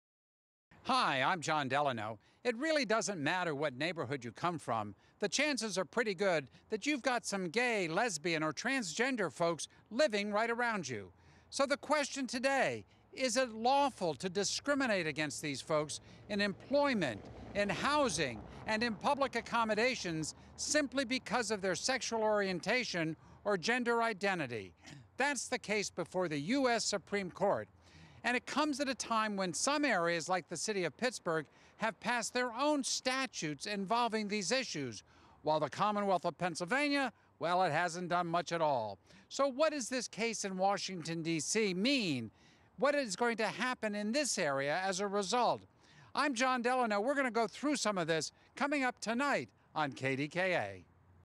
Reporter Update: Jon Delano - LGBT+ SCOTUS Decision
On the day of a major Supreme Court case, KDKA's Jon Delano reports on the political differences of local, state, and federal jurisdictions when it comes to the firing of LGBT+ employees.